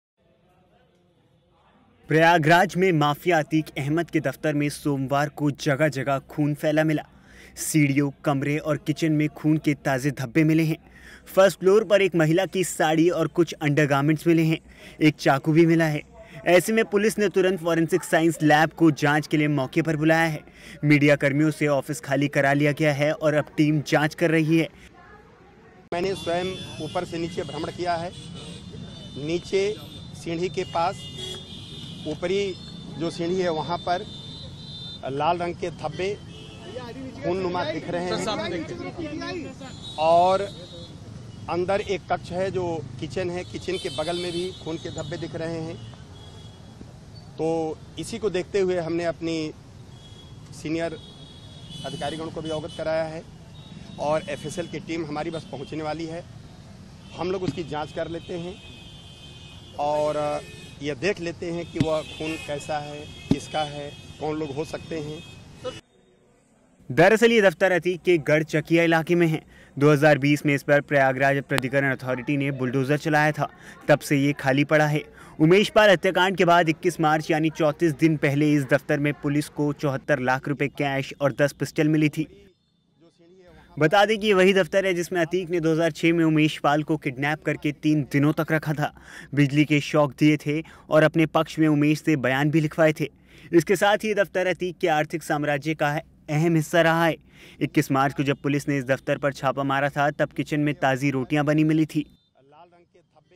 प्रयागराज में Atique Ahmed के कार्यालय में मिला खून.. महिला की साड़ी, अंडरगारमेंट्स, चूड़ियां भी बरामद
Video Courtesy: IANS TV